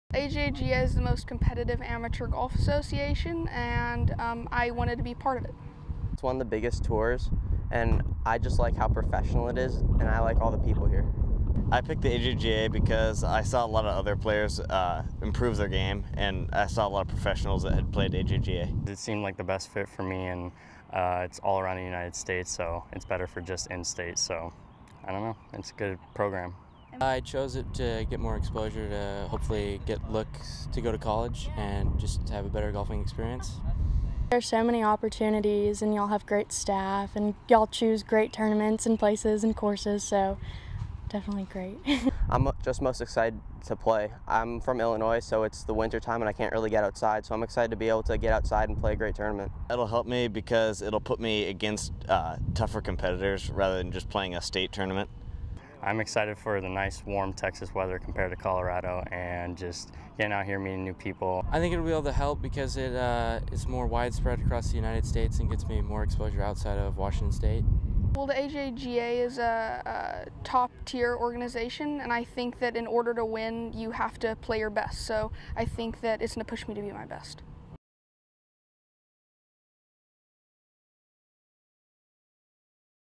"The AJGA's the most competitive amateur golf association." First-time players on choosing the AJGA
We asked players at a few of our Preview Series events to talk about why they chose the AJGA and what they are looking to get out of the experience. Our Preview Series events prioritize first-time players and give them the opportunity to compete at the national level.